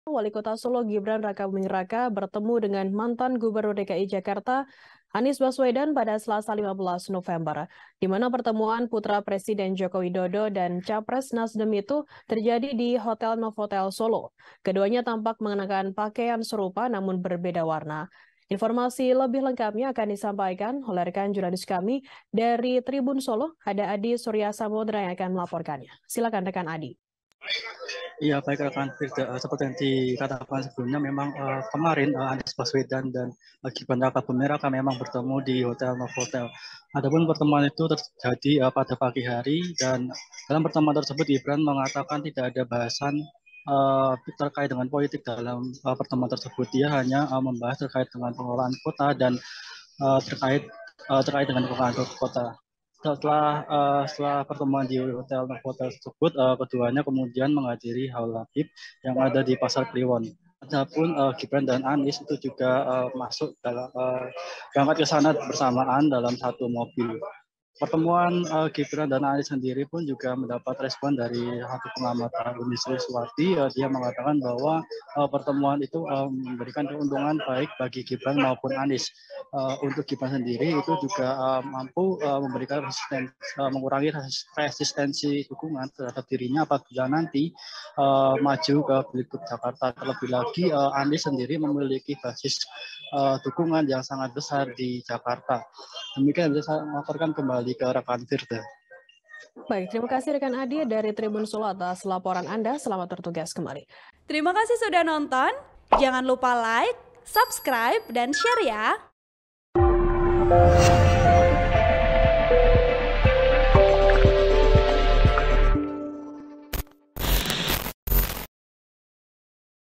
Eks Gubernur DKI Jakarta Anies Baswedan Temui Walkot Gibran Rakabuming Raka di Solo, Bahas Pilpres?
TRIBUN-VIDEO.COM - Wali Kota Solo, Gibran Rakabuming Raka bertemu dengan mantan Gubernur DKI Jakarta, Anies Baswedan, Selasa (15/11/2022).

Di mana pertemuan putra Presiden Joko Widodo (Jokowi) dan Capres NasDem itu terjadi di Hotel Novotel Solo.

Keduanya tampak mengenakan pakaian serupa tapi berbeda warna. (*)